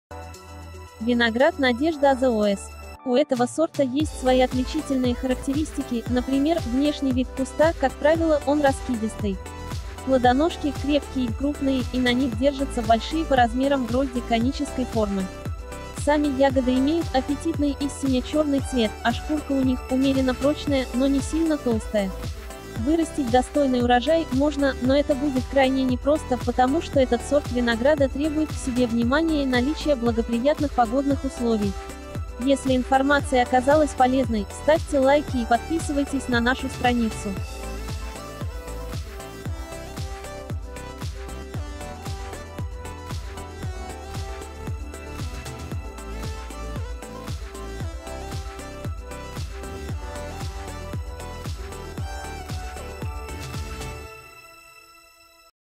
Виноград Надежда АЗОС
У этого сорта есть свои отличительные характеристики, например, внешний вид куста (как правило, он раскидистый).

Плодоножки – крепкие и крупные, и на них держатся большие по размерам грозди конической формы. Сами ягоды имеют «аппетитный» иссиня-чёрный цвет, а шкурка у них – умеренно прочная, но не сильно толстая.

Вырастить достойный урожай – можно, но это будет крайне непросто, потому что этот сорт винограда требует к себе внимания и наличия благоприятных погодных условий.